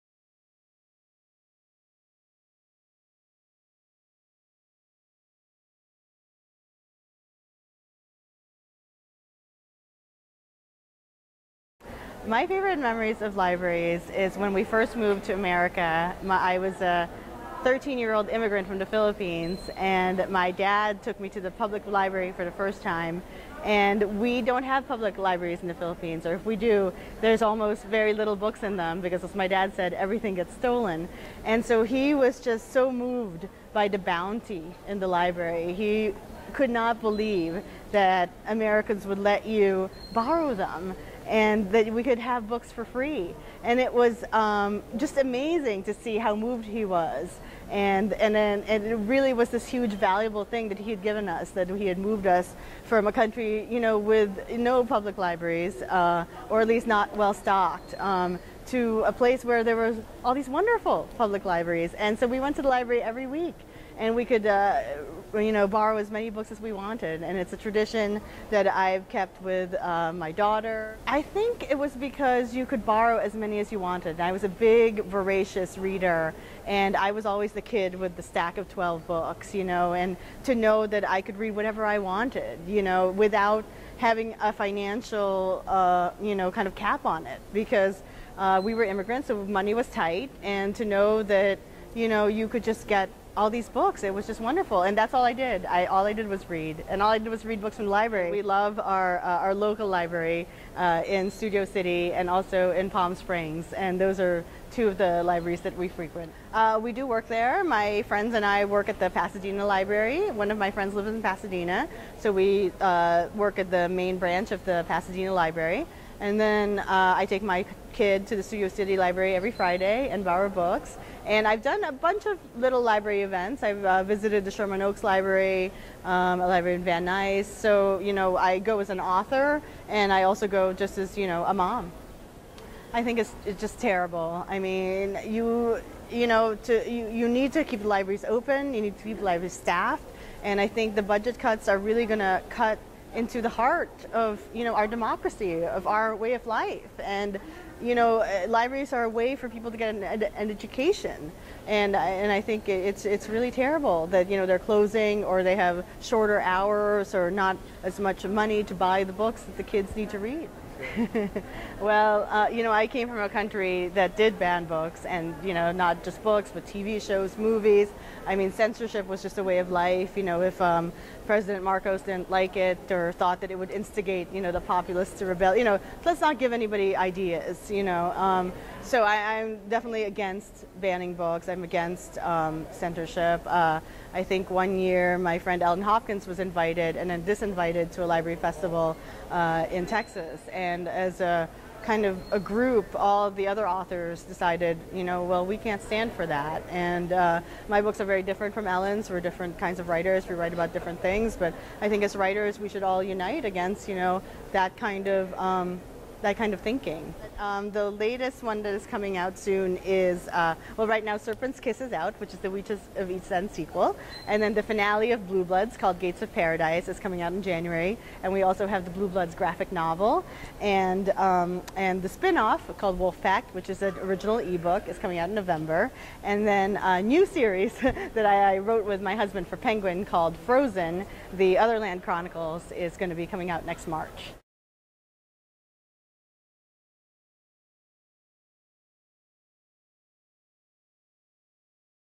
Melissa de la Cruz
Author Melissa de la Cruz shares her experiences with libraries and talks about the importance of libraries to Democracy.